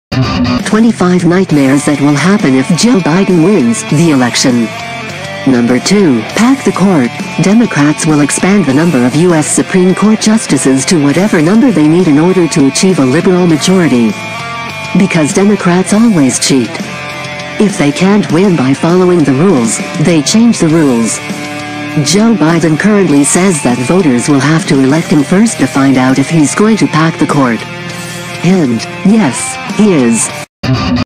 #2 Pack the Court - 25 Nightmares that DID Happen
Democrats will expand the number of US Supreme Court justices to whatever number they need in order to achieve a liberal majority. Because Democrats always cheat. If they can’t win by following the rules, they change the rules! Joe Biden currently says that voters will have to elect him first to find out if he’s going to pack the court. (Hint: Yes, he is.)